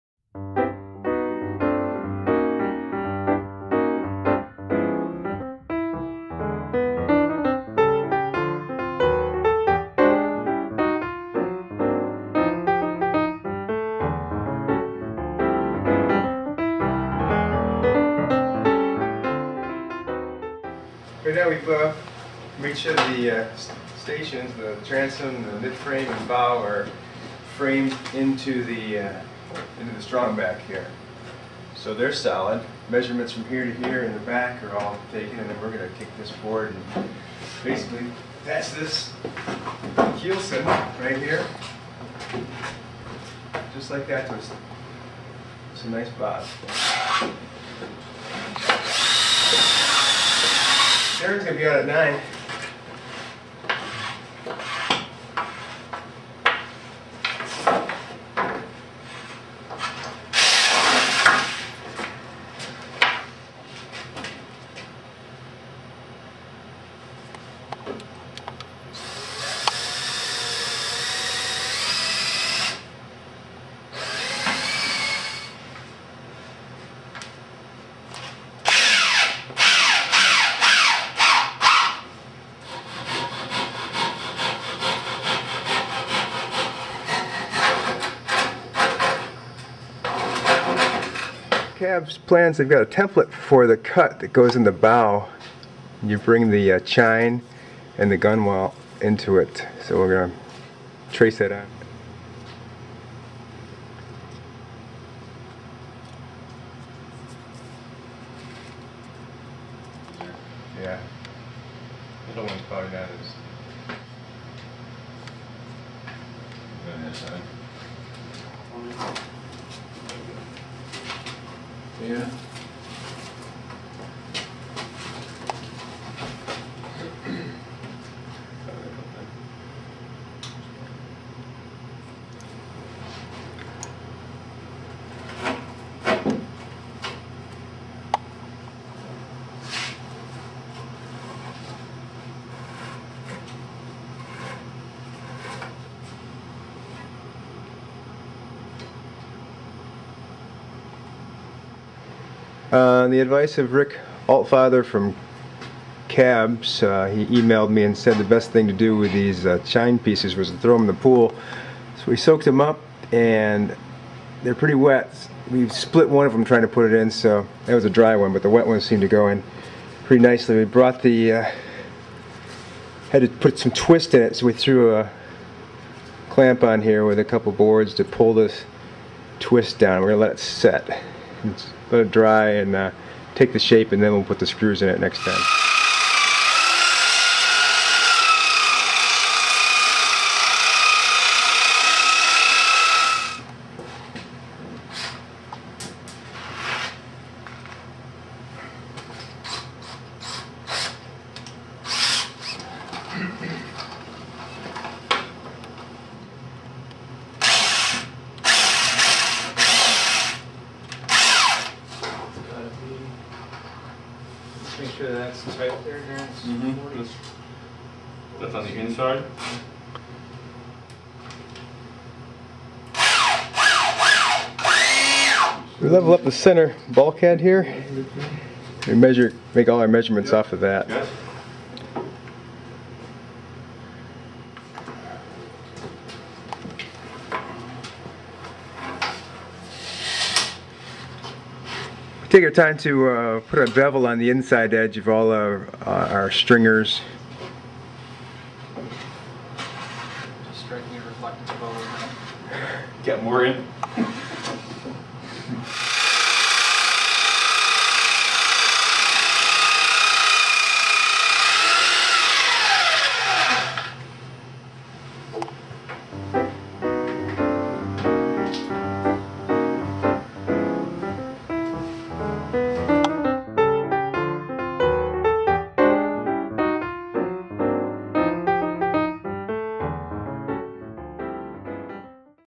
Building a plywood Optimist Pram (dinghy) - Part 4
Using plans from the Cleveland Amateur Boatbuilding and Boating Society (CABBS), a modern equivalent to the original Clark Mills designed Optimist, we continue to build three prams at once. In this video we true-up the three stations - bow, mid-frame, transom, and add the keelson, chine, and other stringers. Once these are in place we can add the plywood skin to our boats.